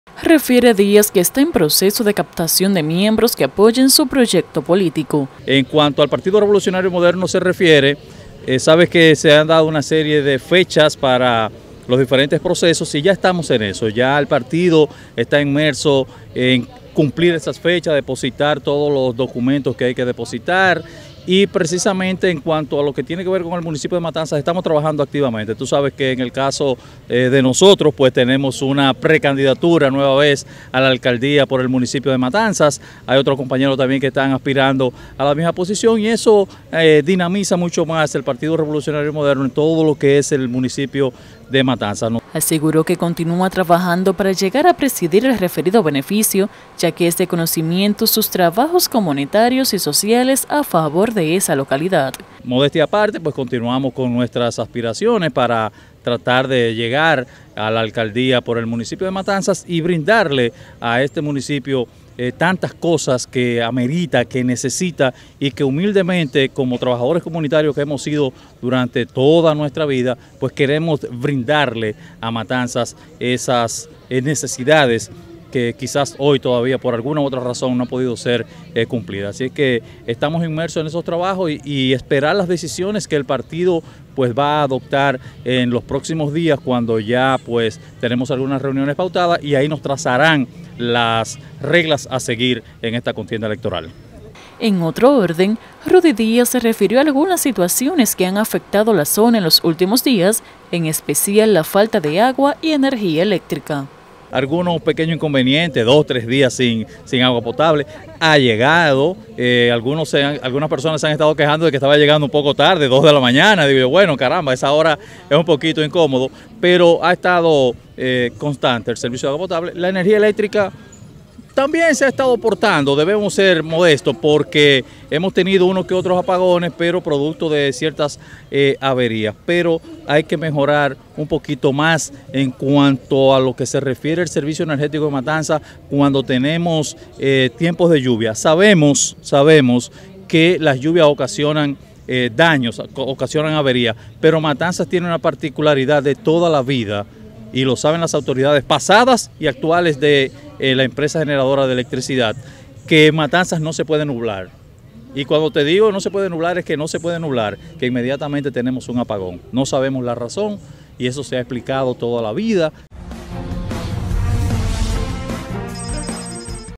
Avanza proyecto político de aspirante a alcaldía de Matanzas
El precandidato a alcalde por el municipio Matanzas, Ruddy Díaz, aseguró que trabajan de manera ardua en los procesos políticos para sus aspiraciones y la de los candidatos del PRM.